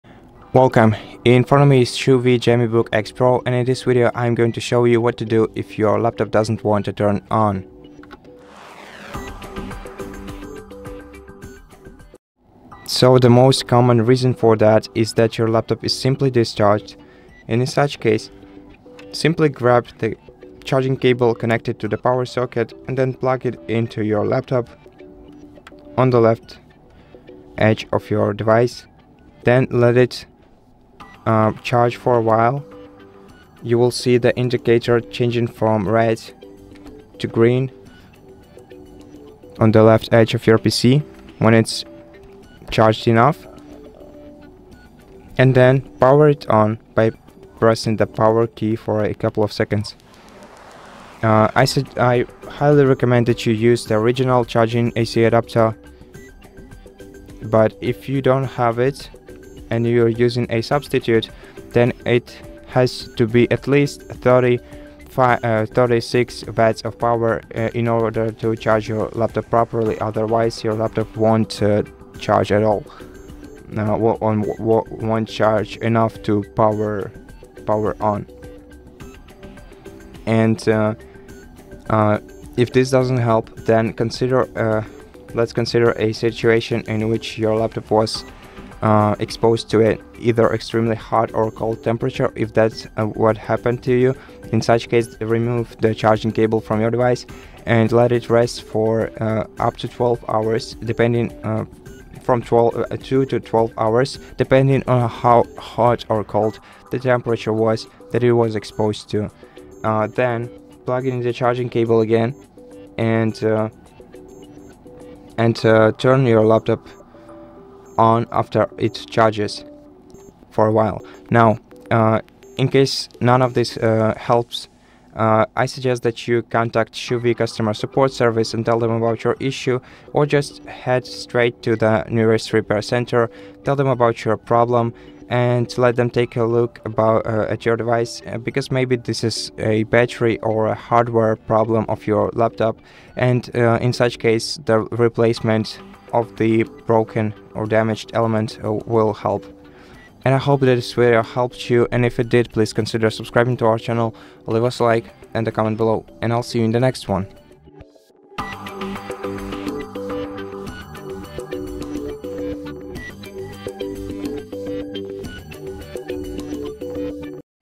CHUWI GemiBook X Pro Not Turning On – How to Fix Power Issues
If your CHUWI GemiBook X Pro laptop is not turning on, this video will help you troubleshoot and solve common power problems. We explain step-by-step what to do if your GemiBook X Pro won't start, including checking the battery, using the correct charger, and what to do if your laptop was exposed to extreme temperatures. Learn how to identify charging issues, understand the indicator lights, and when to contact CHUWI support or visit a repair center. This guide is perfect for anyone experiencing power issues with their CHUWI GemiBook X Pro and looking for easy solutions to get their device working again.

How to fix CHUWI GemiBook X Pro not turning on?
How to charge CHUWI GemiBook X Pro if it won't start?
What to do if CHUWI GemiBook X Pro power button is not working?

0:00 Introduction and common reasons for not turning on
0:18 Checking and connecting the charger
0:40 Understanding charging indicator lights
1:00 Using the correct AC adapter
1:20 What to do after exposure to extreme temperatures
2:01 When to contact CHUWI support or visit a repair center